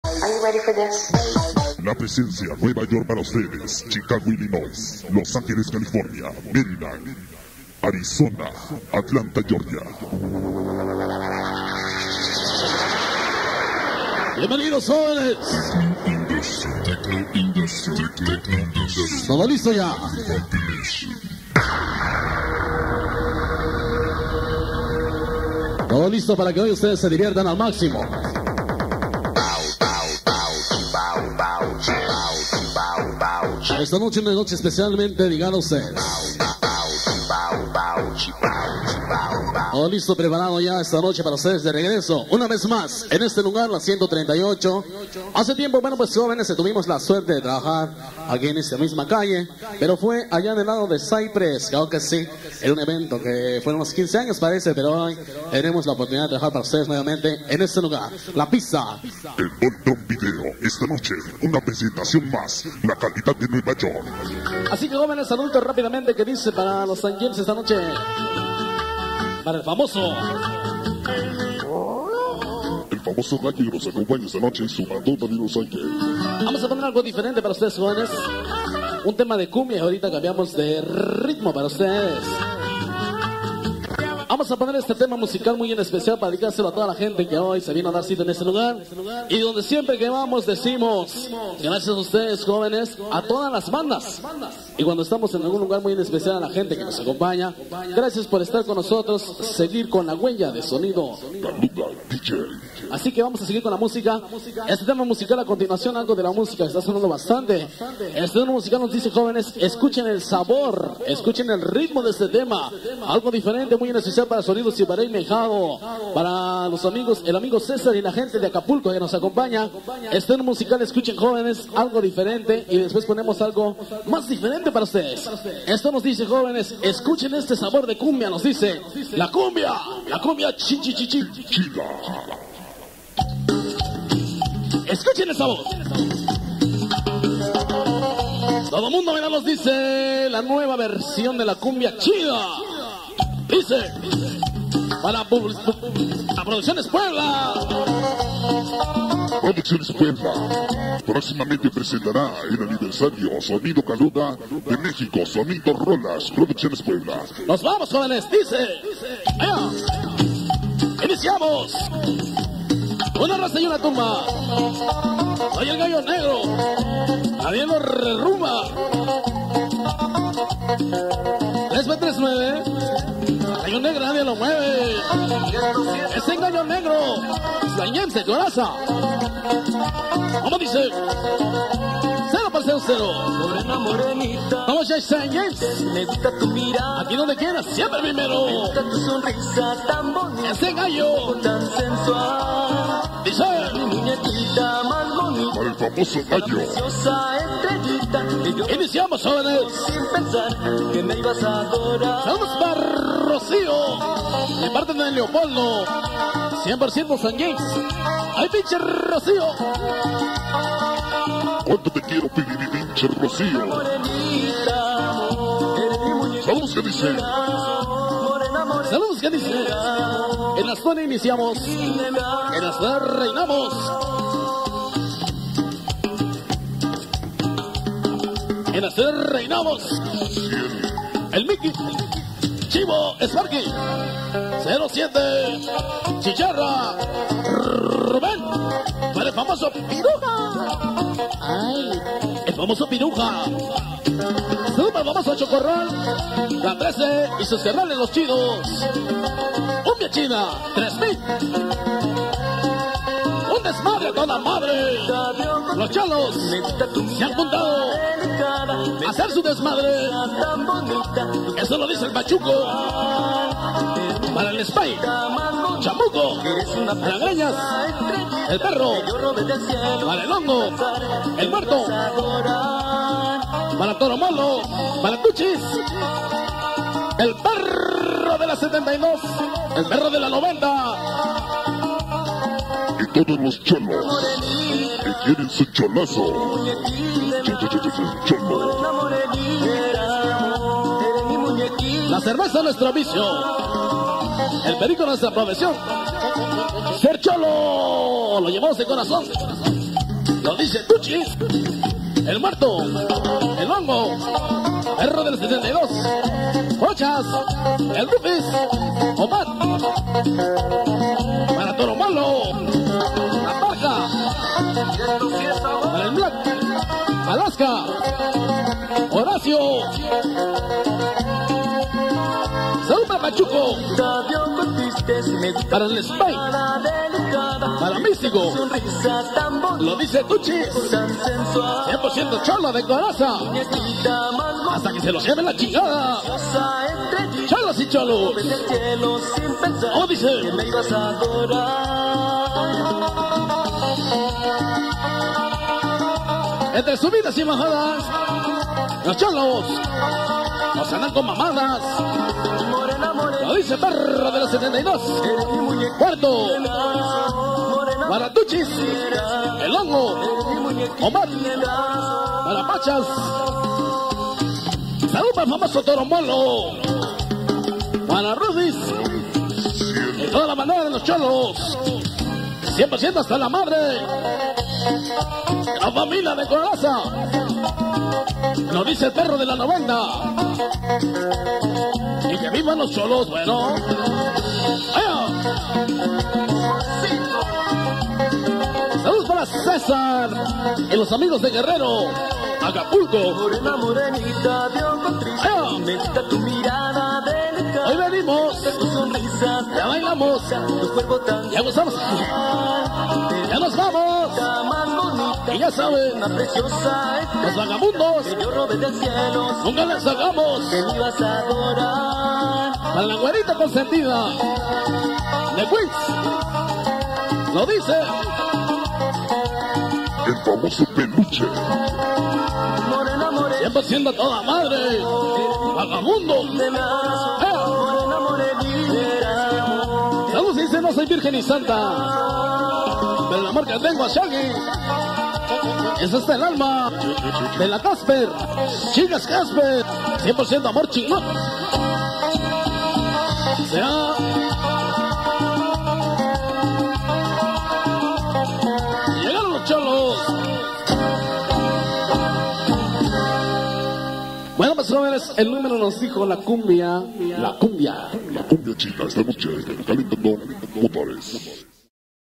SONIDO CALUDA NY 1999
PRESENTACION EN EL BOLTRON VIDEO BRONX NY DAVID HUERTA SONIDO CALUDA 1999